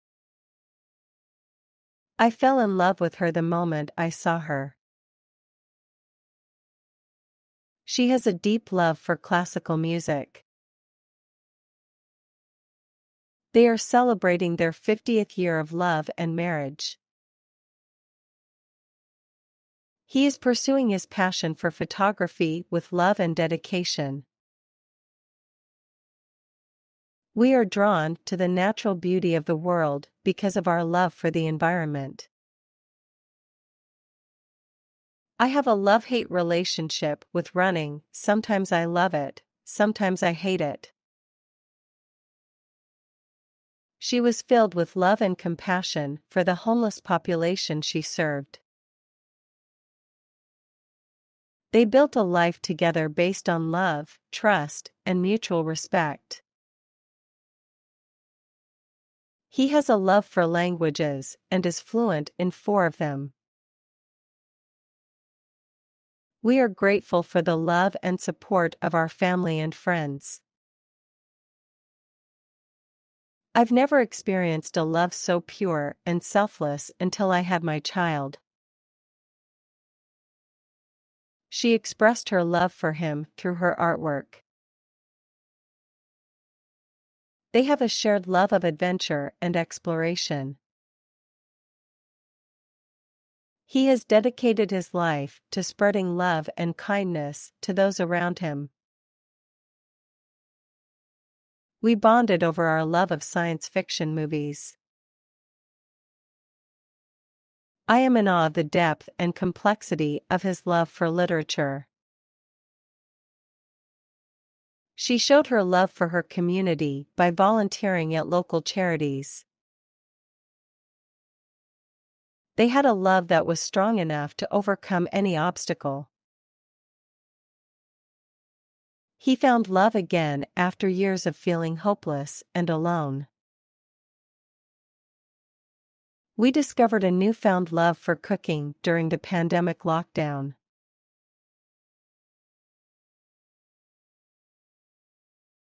Love in sentence - Advanced Level
Examples of using the word love in sentences.